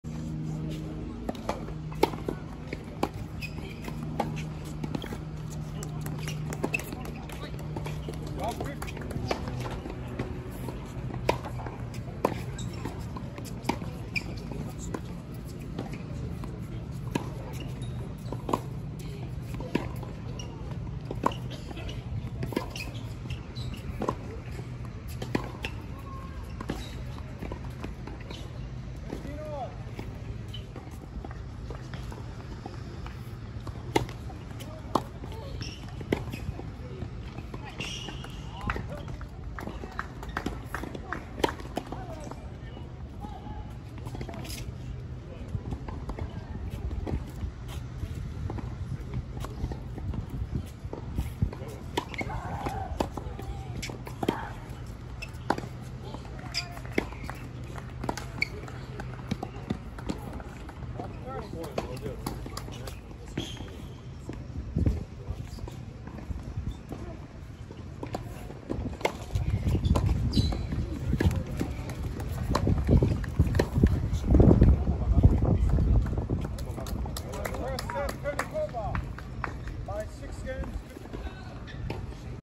Shabashkevich EH G16s final
Katerina Shabashkevich was defeated by Kristina Penickova 6-2, 6-4 in the Eddie Herr International Championships G16s final, December 3, 2023, IMG Academy, Bradenton FL